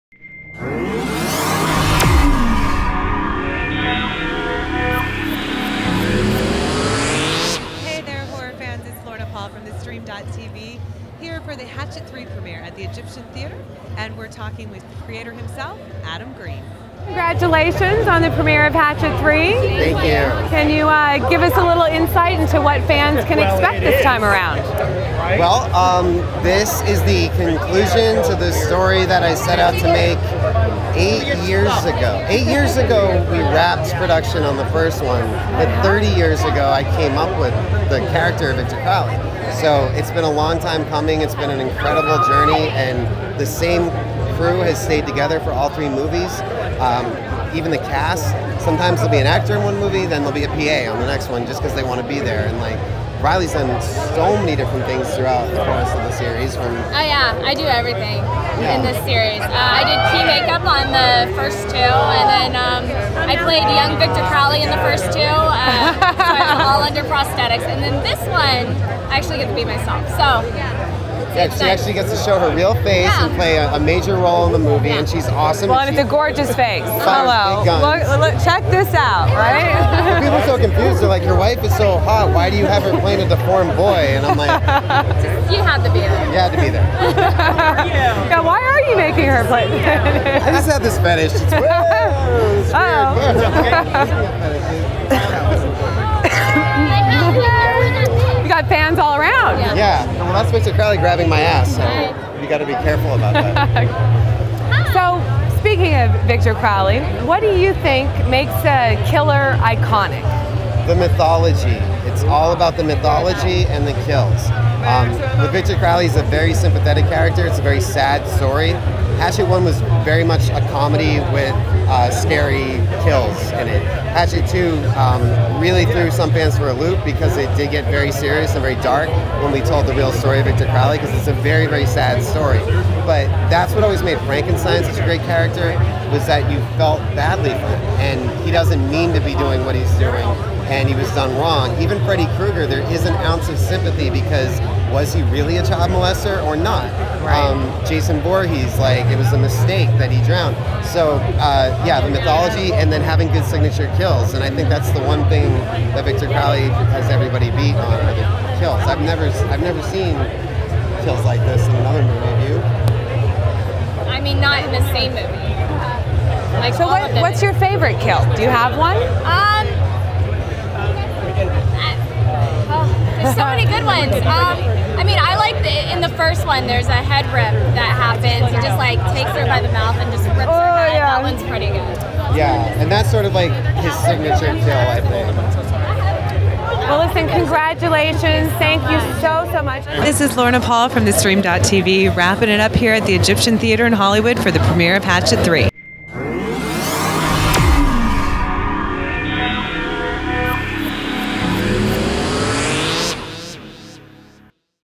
Adam Green - Writer of Hatchet III and Rileah Vanderbilt "Dougherty" Hatchet 3 Premiere
Lorna Paul from theStream.tv interviews writer Adam Green and Rileah Vanderbilt "Dougherty" on the Red Carpet at the premiere of Hatchet III at the Egyptian Theatre in Hollywood, CA.